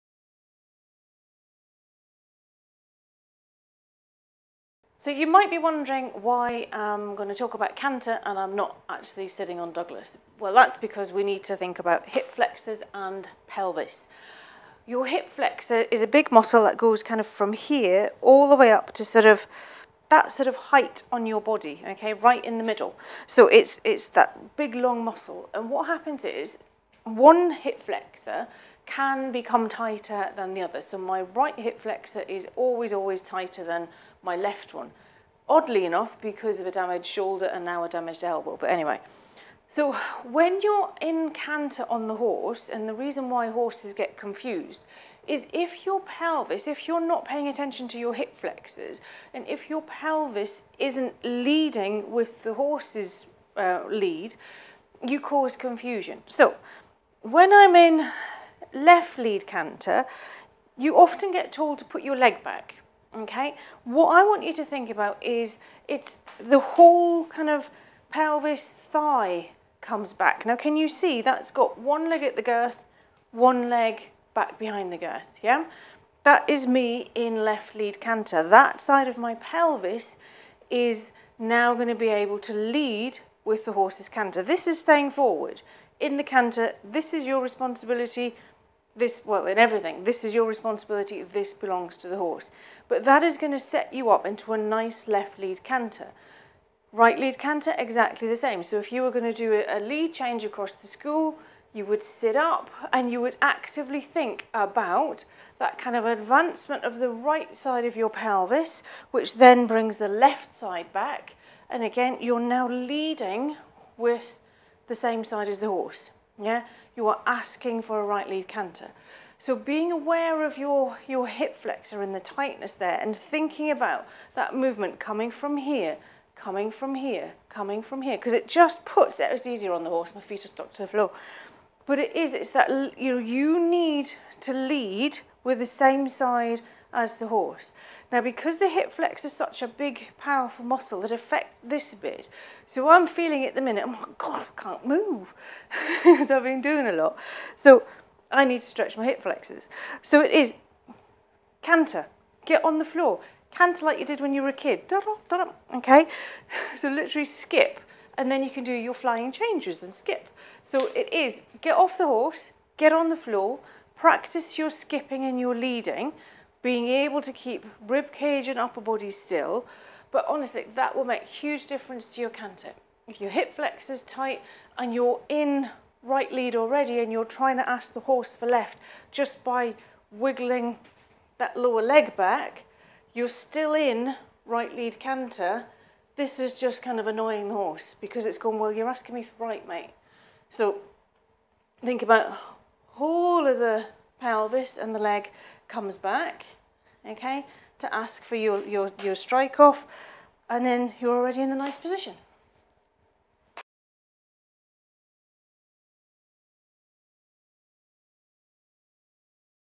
I struggle to get canter on one rein - what can I do?
You may struggle to get canter on one rein, you may struggle with canter strike offs full stop.  Being aware of your pelvis position can help.  And you don't need a ponio to practice!